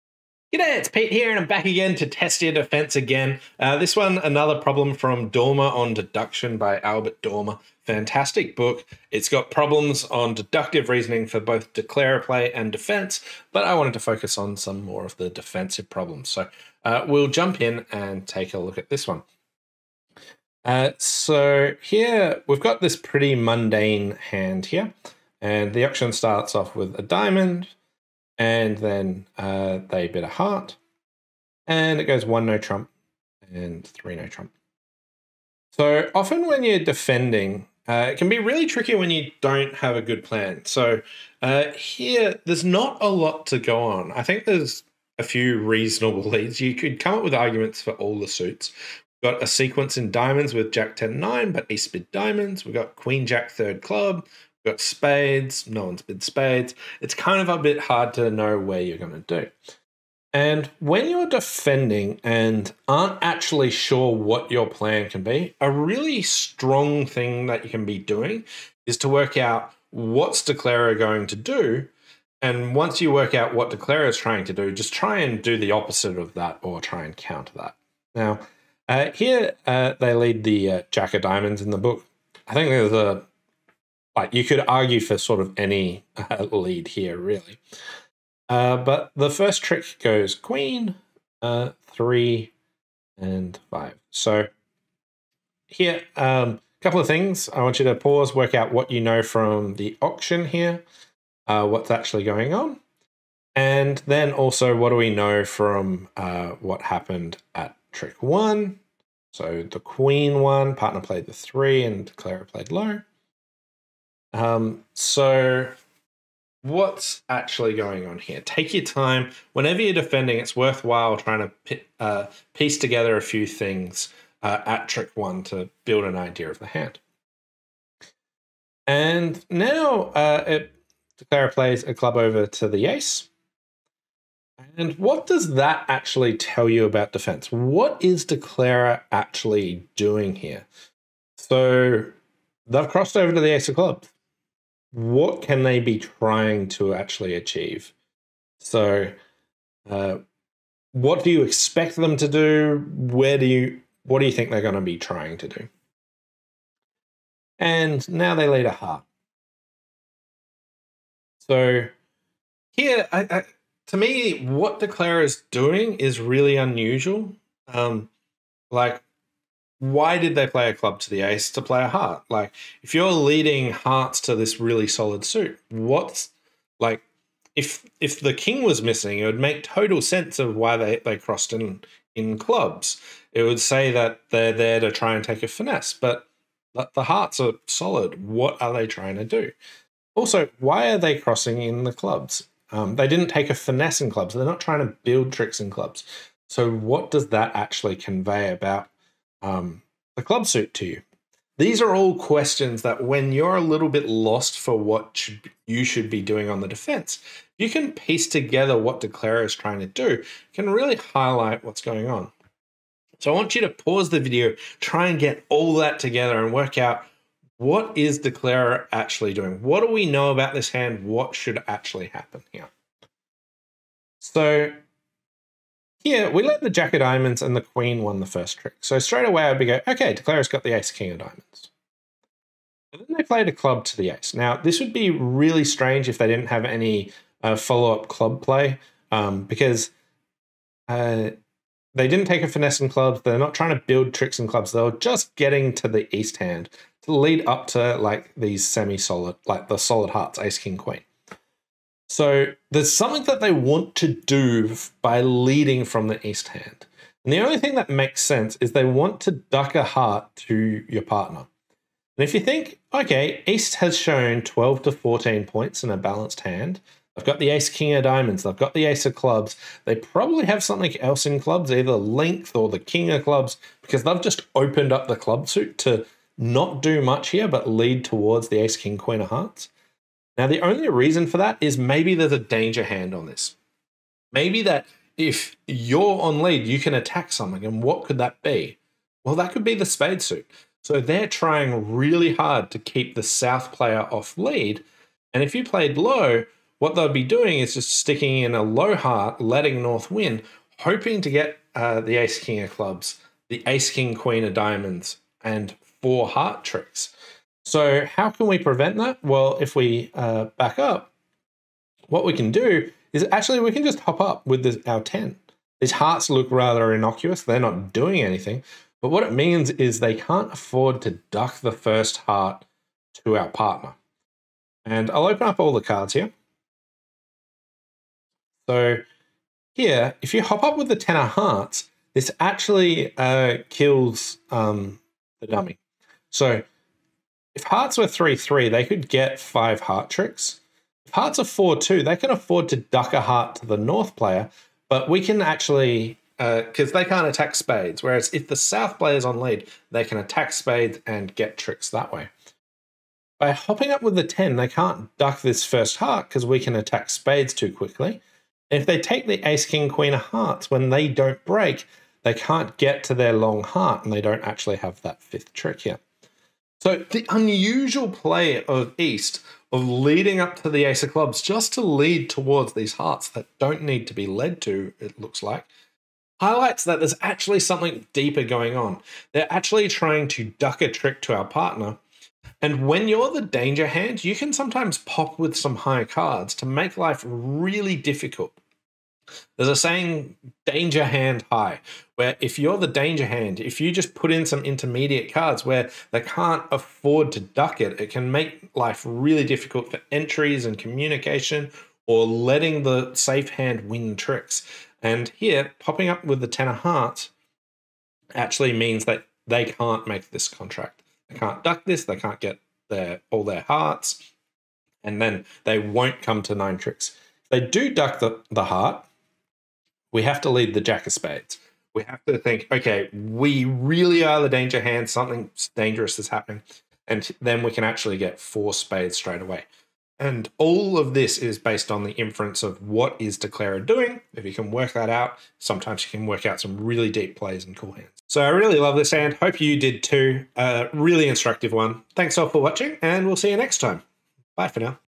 Declarer is trying to...
This Defensive problem came from the book "Dormer on Deduction" by Albert Dormer